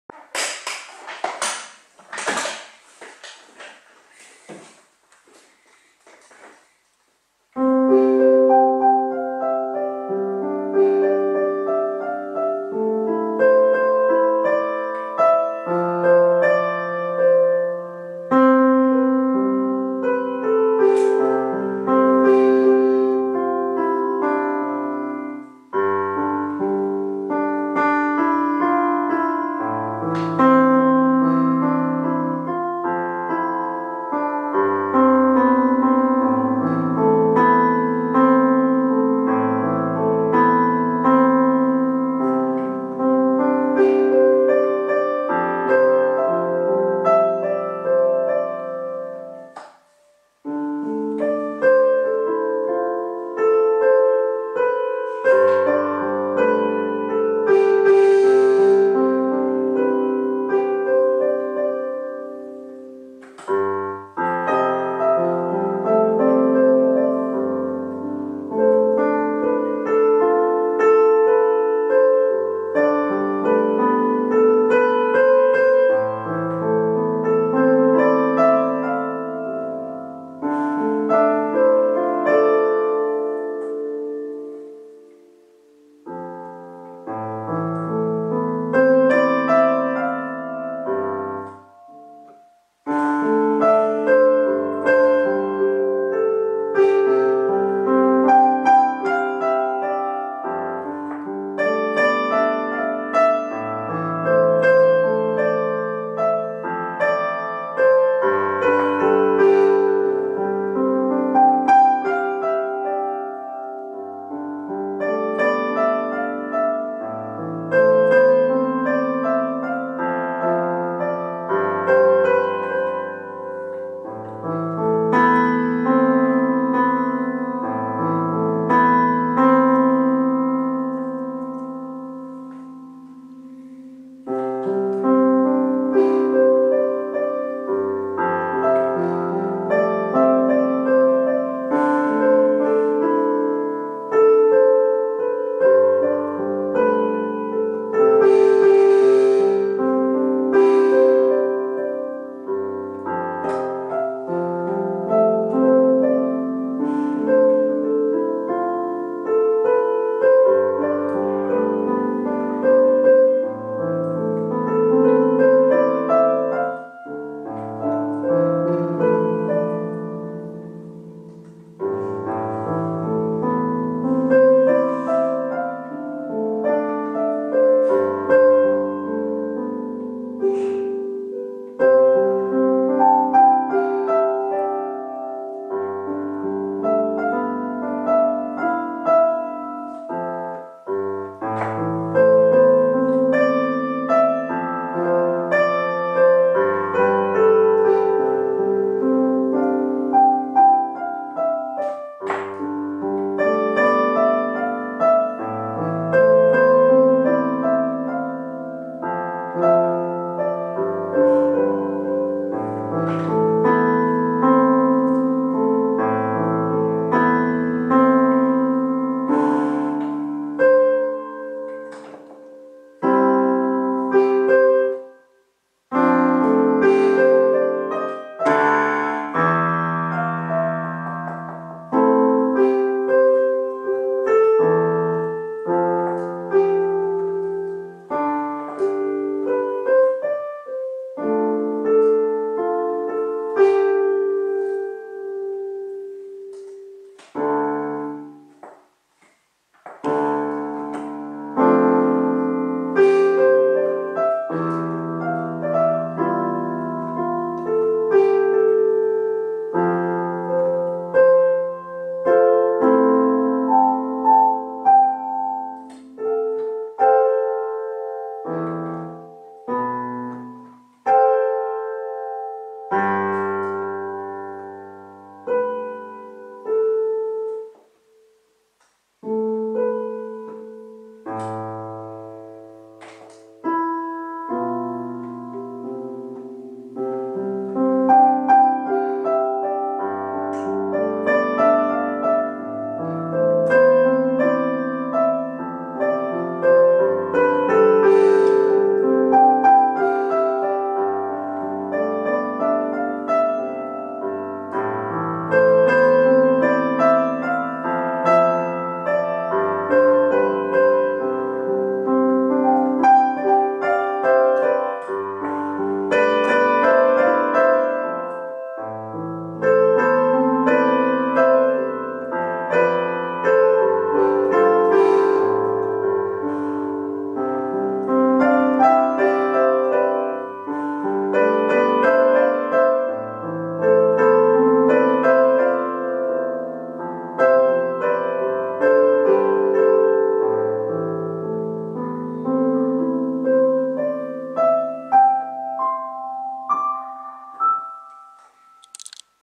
First time reading music with a camera on his back
Hi. This is my first time playing on a sheet music (not sightread but I think it should be). After 3 weeks of practicing, my reading skills have gotten up to something.